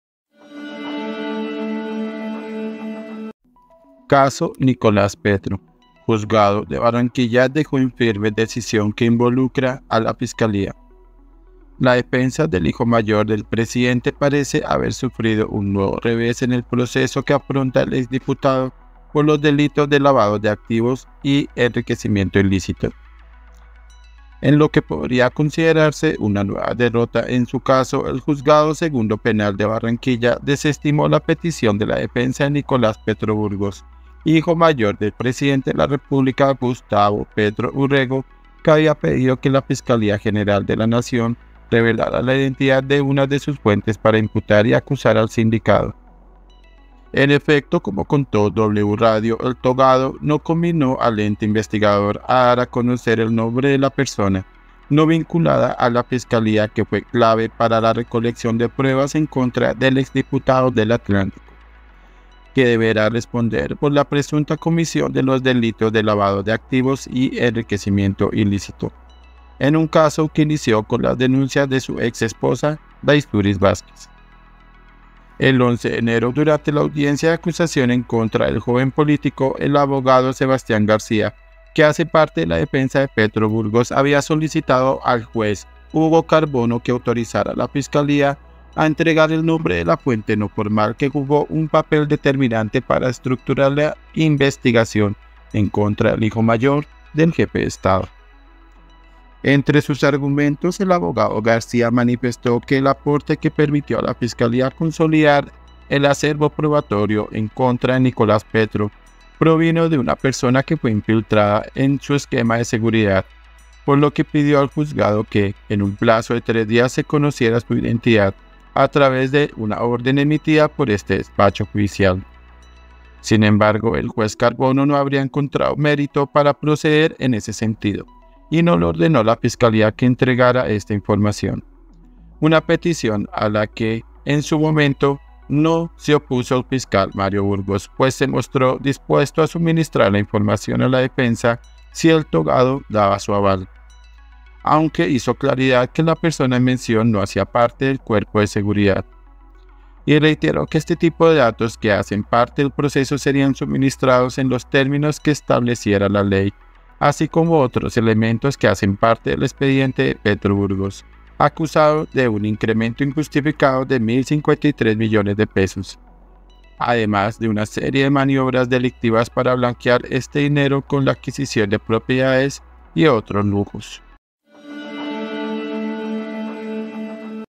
URGENTE 🔴 UN NUEVO REVÉS PARA LA DEFENSA DE NICOLAS PETRO
Caso Nicolás Petro: juzgado de Barranquilla dejó en firme decisión que involucra a la Fiscalía
La defensa del hijo mayor del presidente parece haber sufrido un nuevo revés en el proceso que afronta el exdiputado por los delitos de lavado de activos y enriquecimiento ilícito
INFOBAE